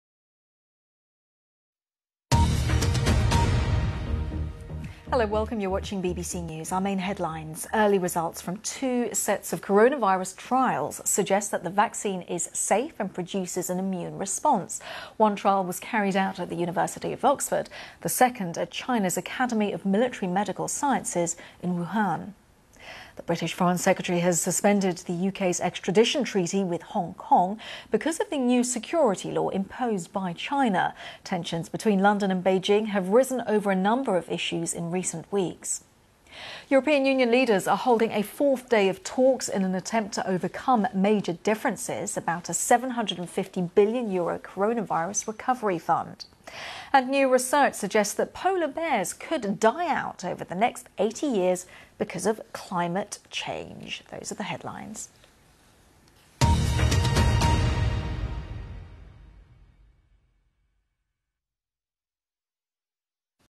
2020 7 21 1 One minute World News
coronavirus vaccine trials; UK-China tensions; EU coronavirus recovery fund; Polar bears warning